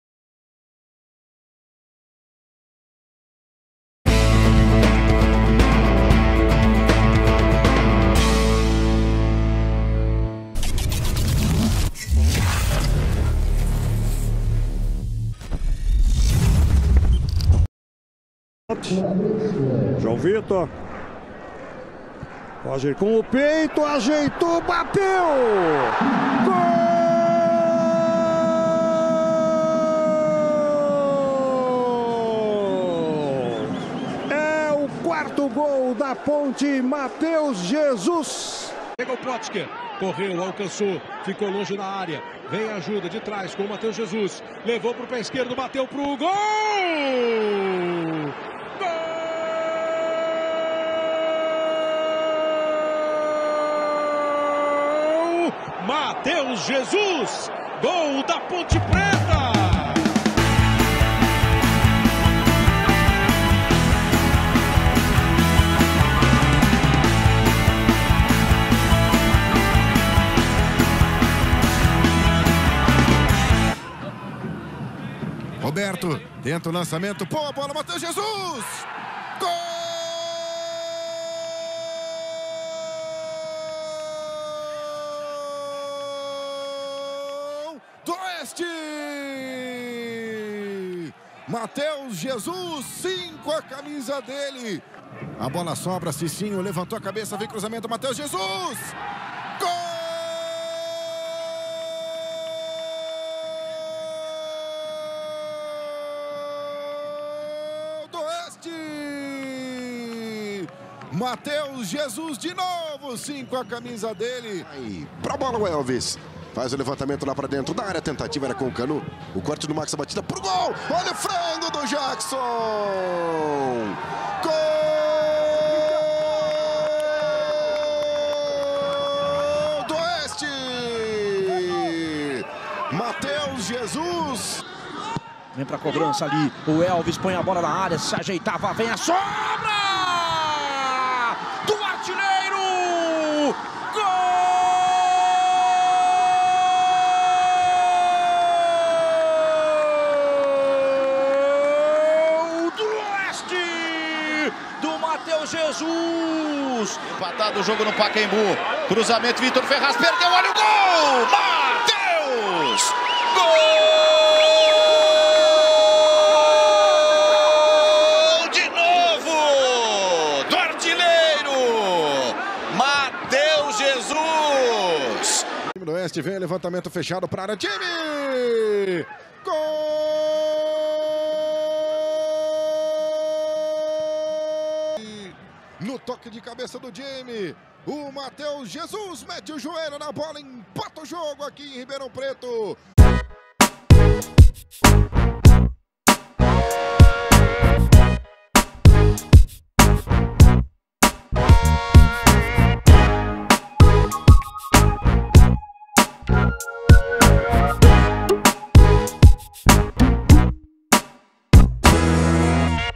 REFORÇO: Volante Matheus Jesus ex Oeste | Gols
Conheça o volante Matheus Jesus, novo reforço do Corinthians. Comente, participe, nos ajude a divulgar.